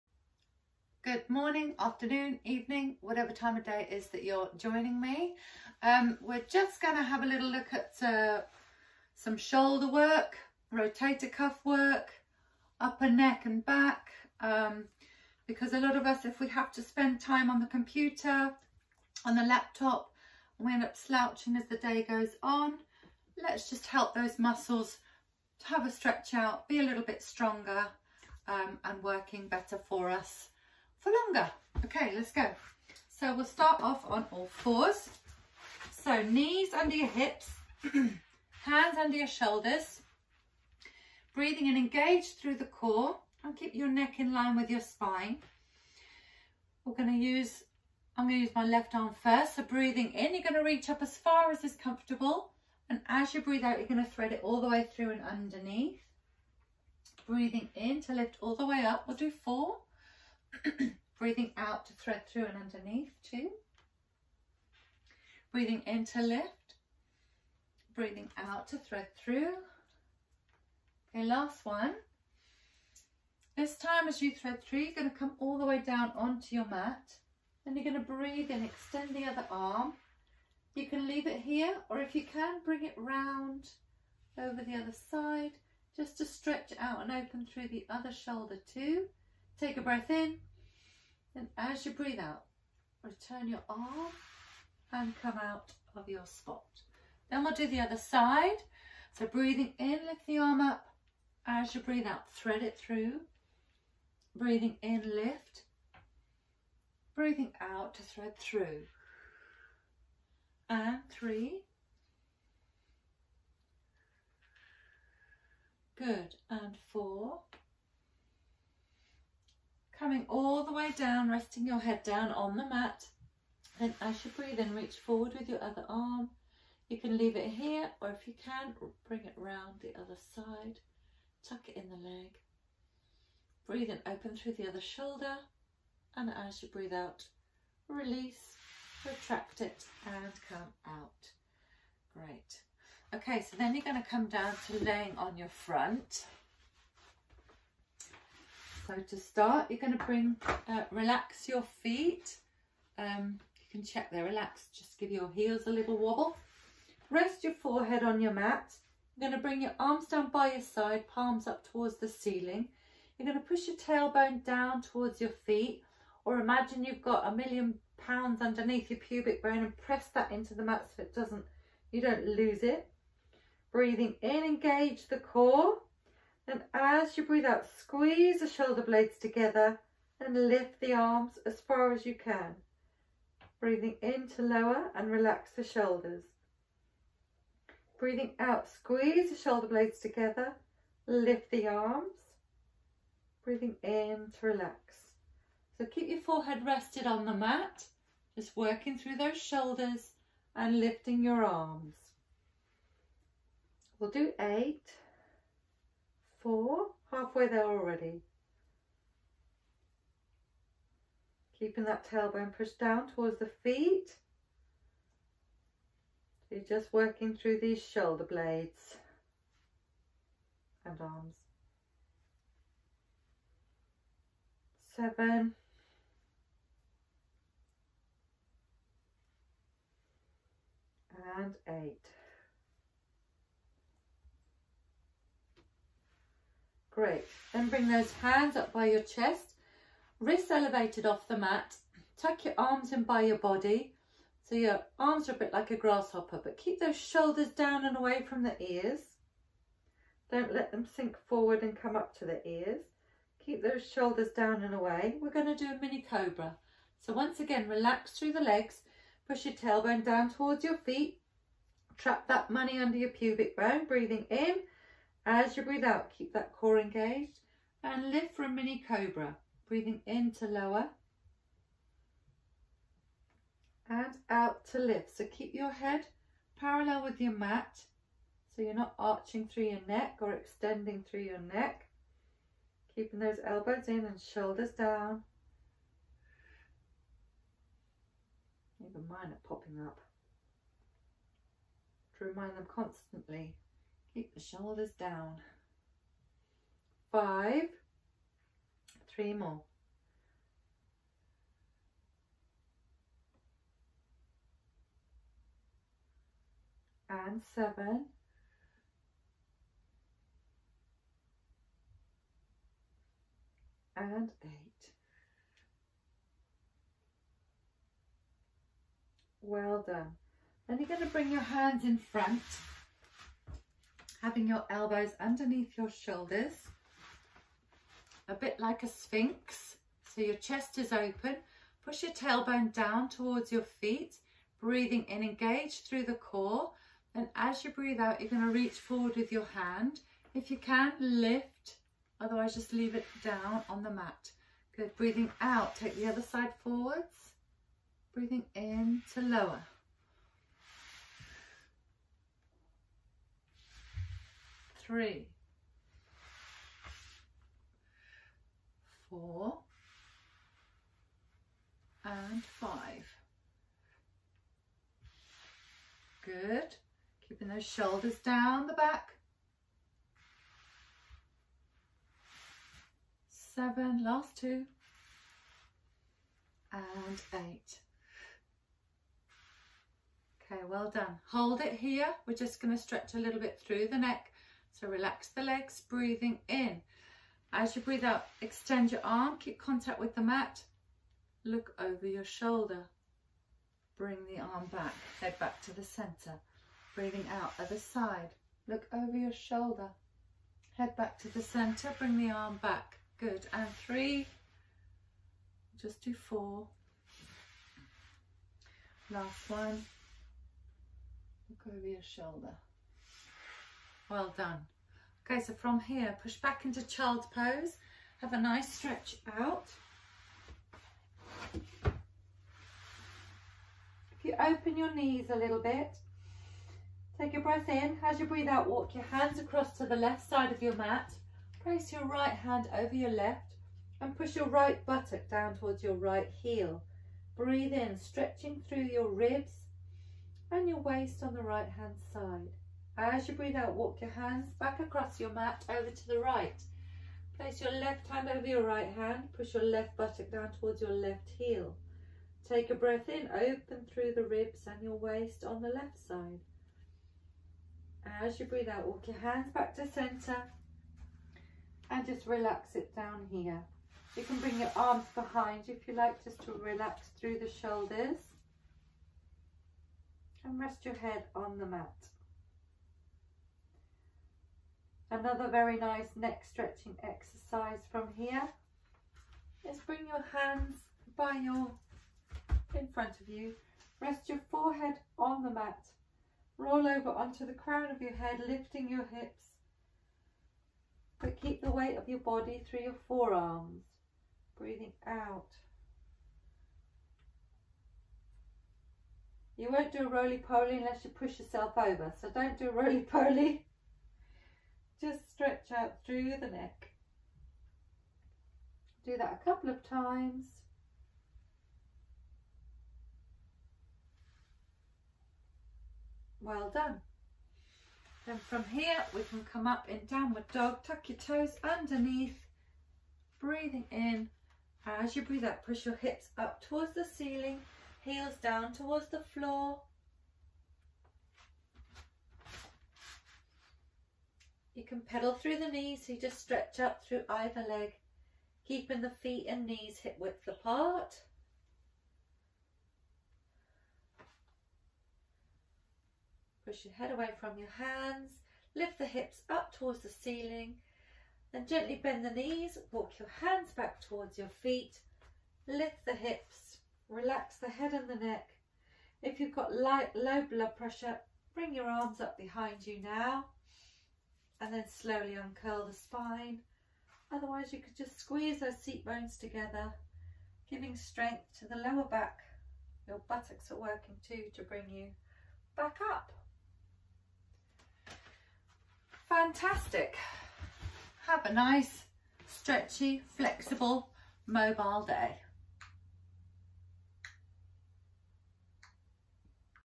Unwind.biz Pilates Rotator Cuffs, shoulders
On All 4’s Rotator cuffs, Prone dart (shoulders and arms only), Sphinx for upper back, back extensors, shoulders, neck stretch, child’s pose, neck lengthen, downward dog, uncurl to standing.
Have a body marvellous day 🤗